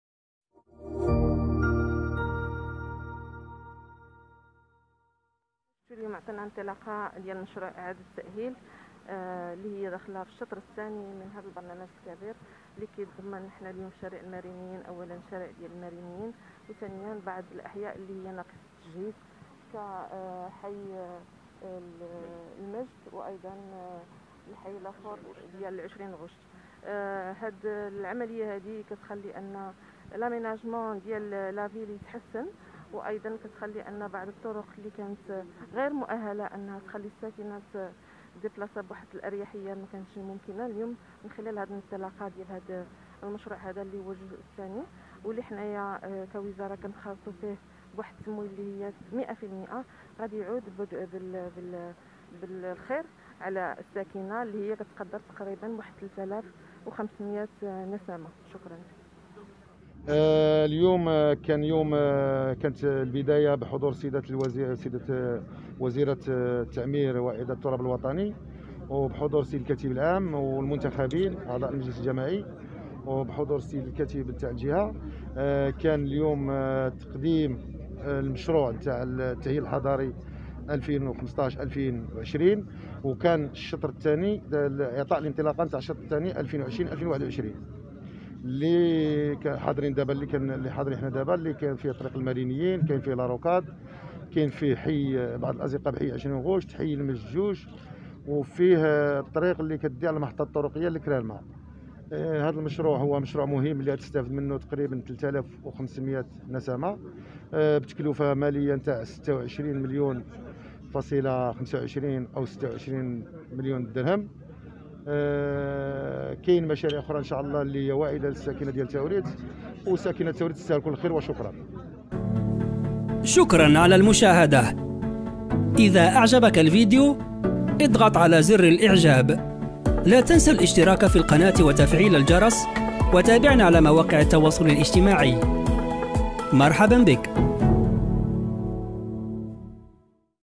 نزهة بوشارب تعطي إنطلاقة الشطر الثاني للتأهيل الحضري لمدينة تاوريرت
2. اضغط على الجرس (🔔) بجانب زر الاشتراك
3. شارك مقاطع الفيديو مع أصدقائك وعائلتك
    يشرفنا اشتراكك بالقناة